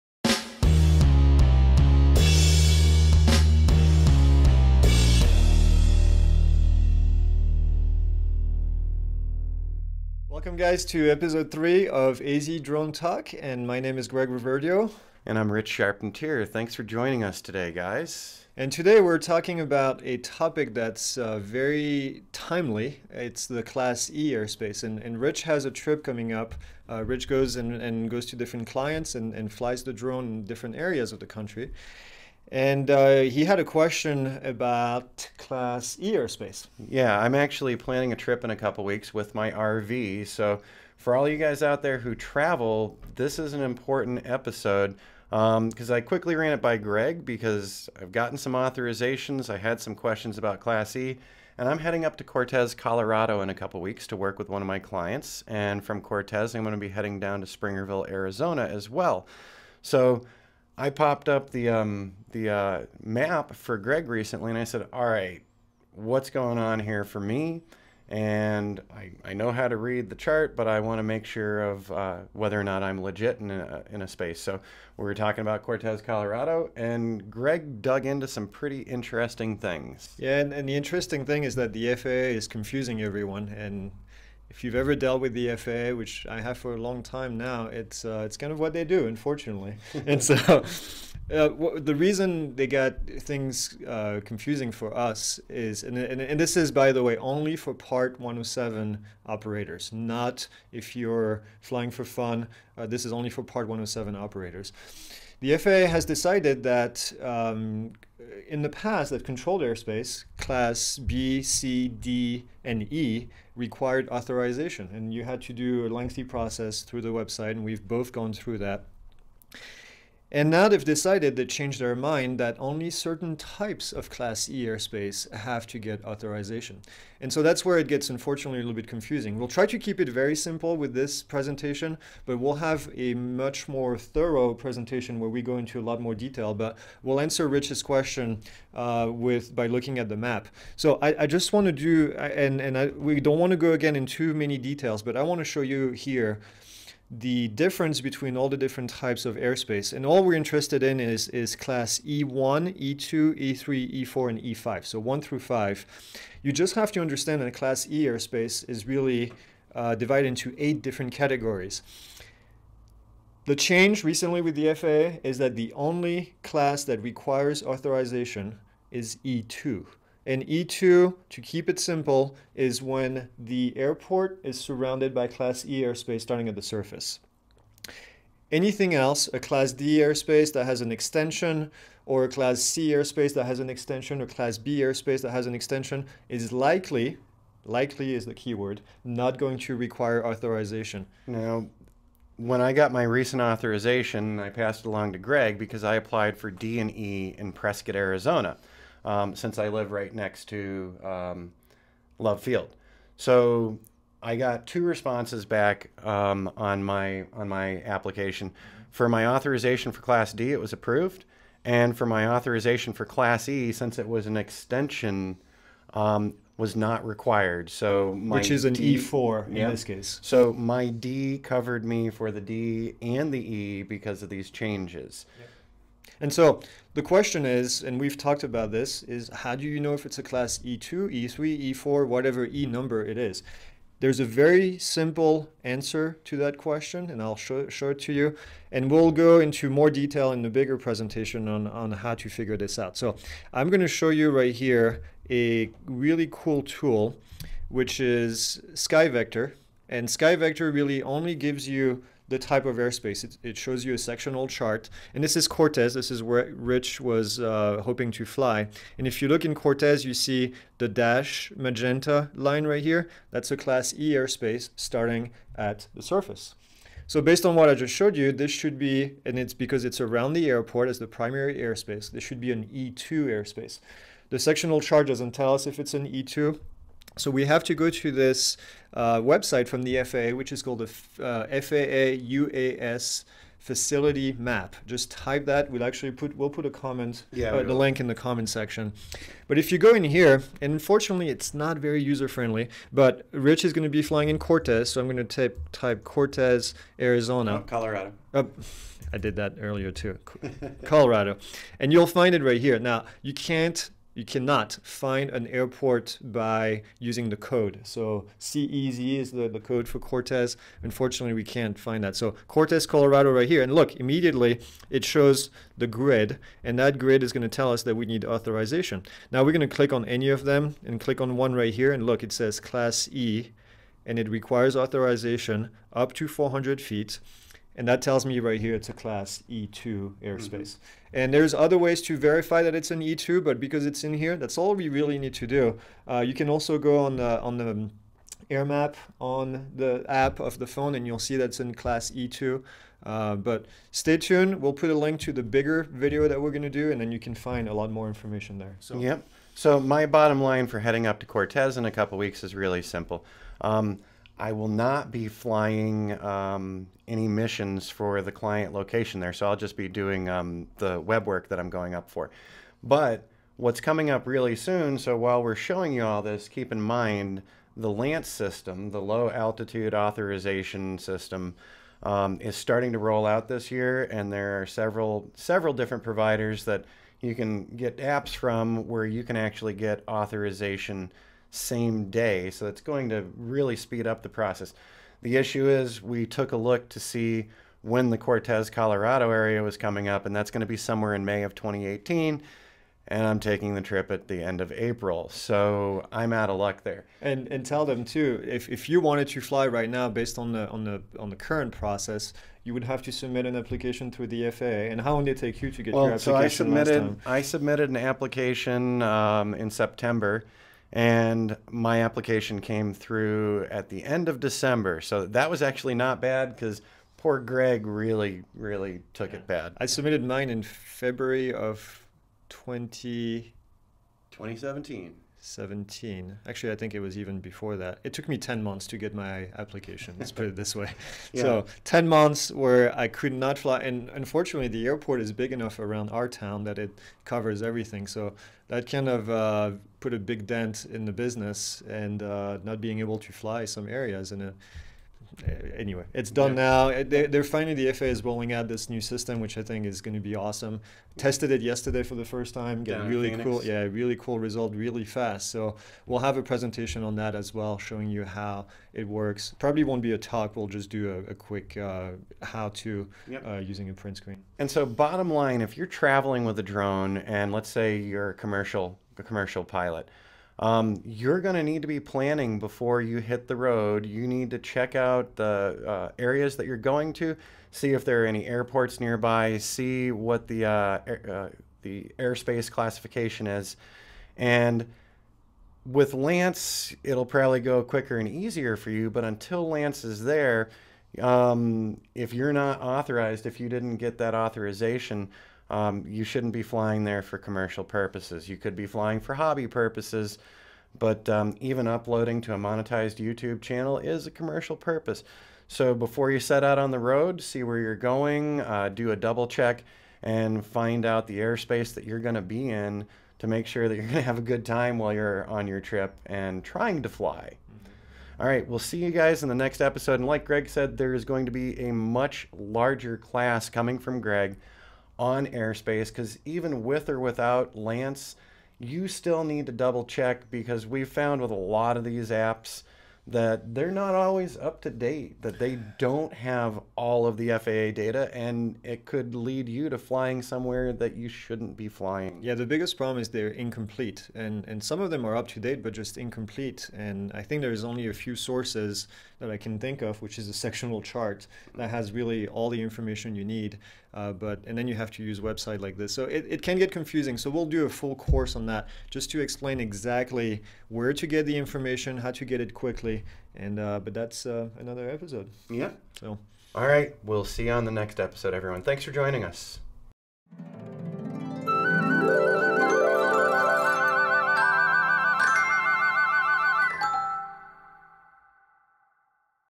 What are the rules for flying in Class E airspace - AZ Drone Talk 003
In this episode, Greg and Rich discuss the recent changes to the FAA approval process concerning class E airspace. They discuss the different levels of class E airspace, and more specifically Class E2. If you are planning to fly as a Part 107 drone operator in class E airspace, be sure to watch this video to make sure you are flying legally.

Train for your FAA Remote Pilot Certificate with Greg's comprehensive test prep course.

Interior VLOG Tripod

Outdoor steady cam